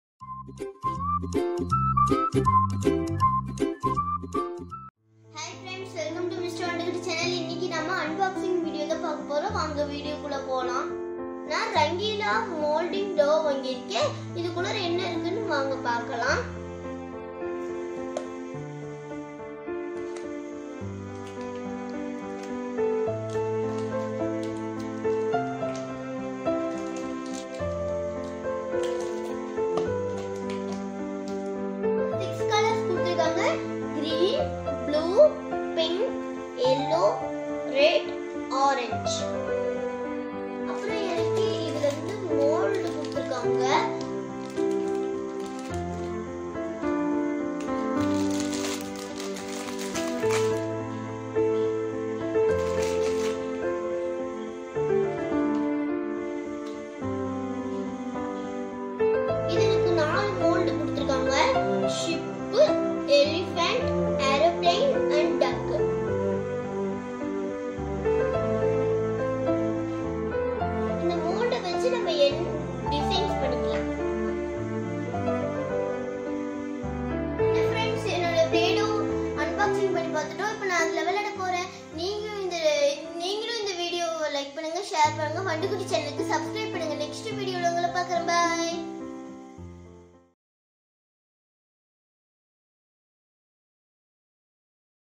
Rangeela Moulding dough Unboxing in Tamil | Mr Vandukutty
This video is about unboxing the Rangeela moulding dough..

keep supporting